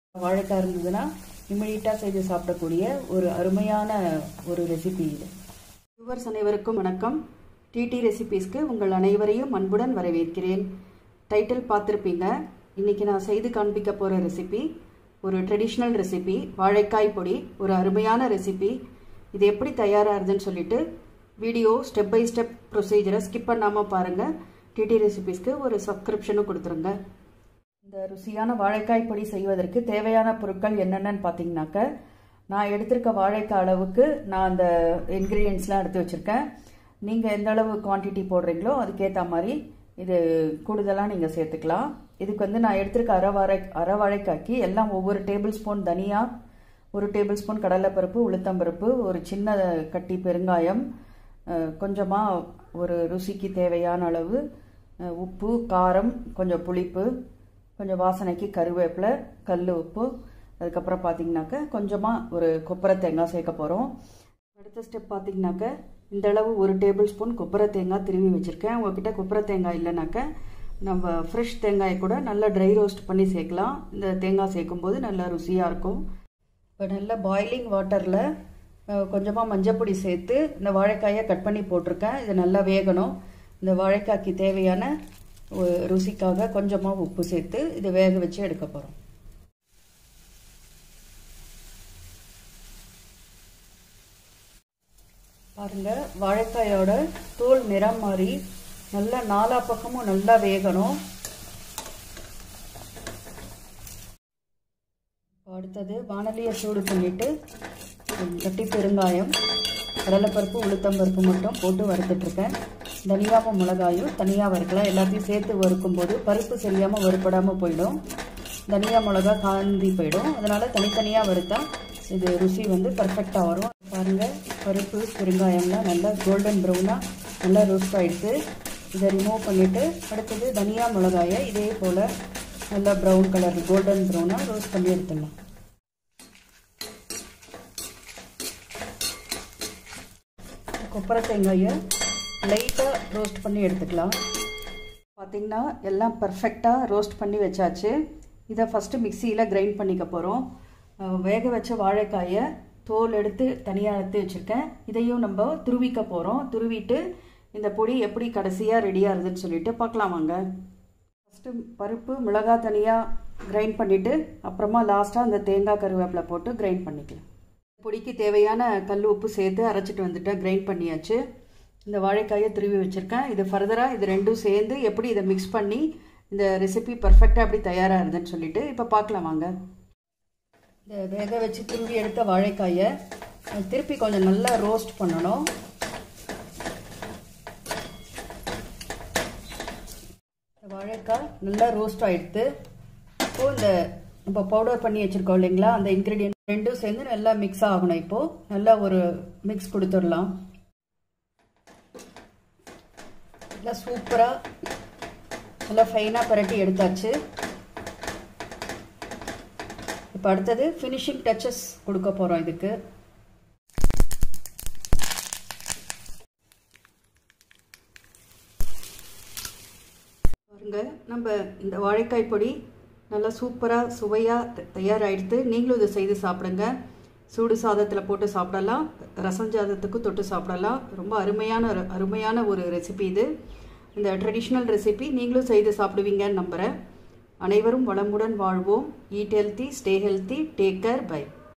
Vaazhakai Podi | வாழைக்காய் பொடி | Raw Banana Spice Powder | Raw Plantain Spice Powder
Vaazhaikaai Podi | வாழைக்காய் பொடி |
Raw Banana Spice Powder
Raw Plantain Spice Powder
Vaazhaikai Podi seyvathu epadi?
How to make Plantain Spice Powder?
How to do Spicy Plantain Crumble?
@T T Recipes ​

Dear viewers
This is a tasty easy spice powder
which can be prepared instantly
with raw plantain. Eat this
Spicy Plantain Crumble
along with hot white rice and
with other south Indian recipes.

try this recipe. Pls feel free to
friends.

for more traditional tasty
recipes.